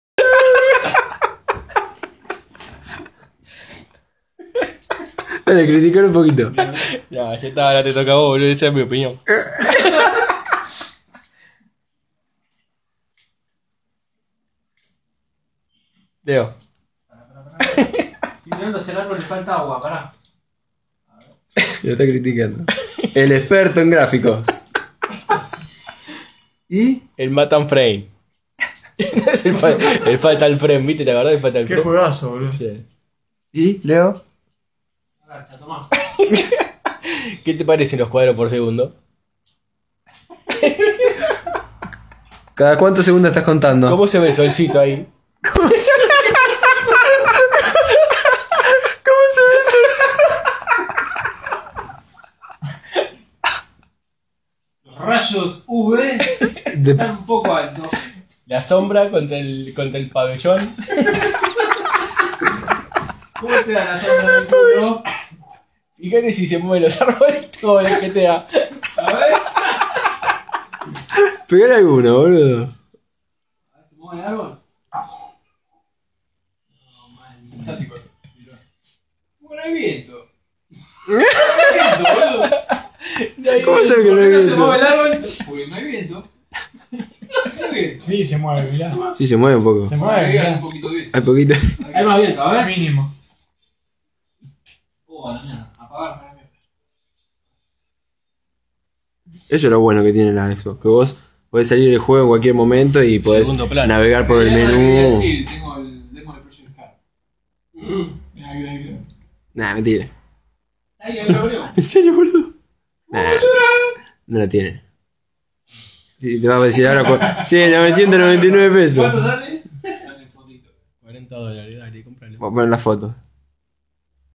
altos graficos segun penny!!!
viendo texturas en los games!! criticas de penny!!